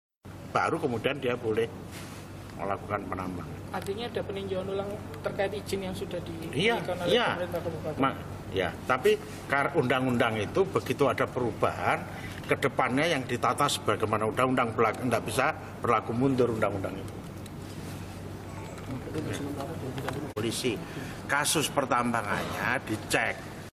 Soekarwo Janji Tutup Penambangan Jika Tak Punya Izin
Gubernur Jawa Timur, Soekarwo berjanji akan menutup usaha penambangan pasir di Desa Selok Awar-awar, Kecamatan Pasiran, Lumajang jika memang usaha penambangan itu tidak memiliki izin dari pemerintah setempat.